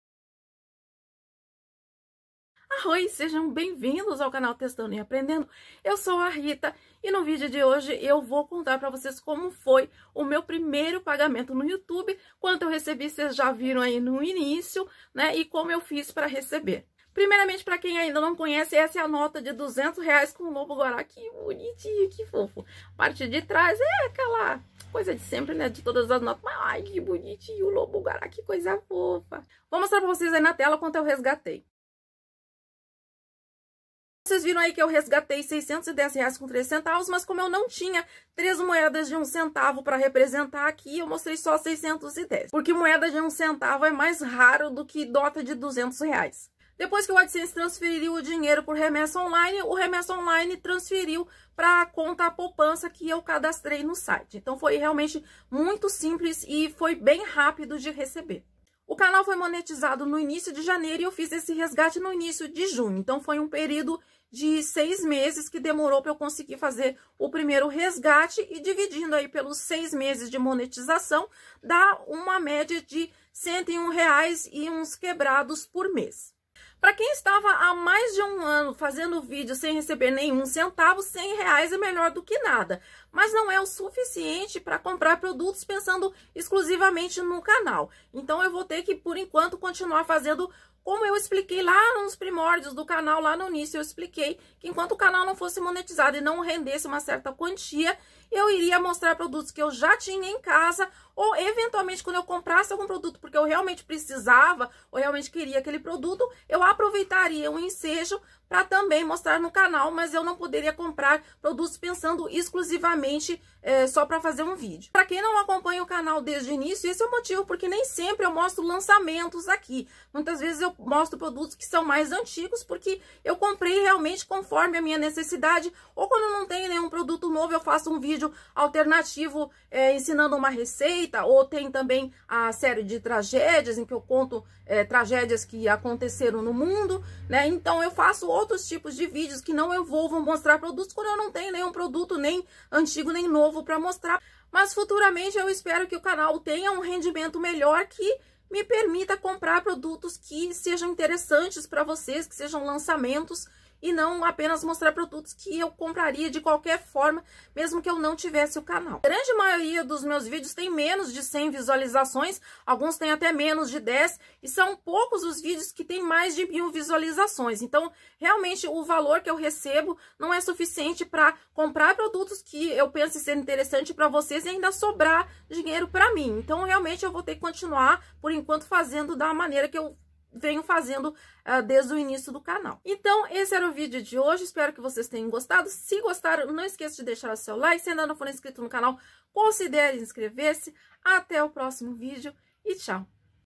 MEU PRIMEIRO PAGAMENTO DO YOUTUBE
Contei quanto recebi e em quanto tempo.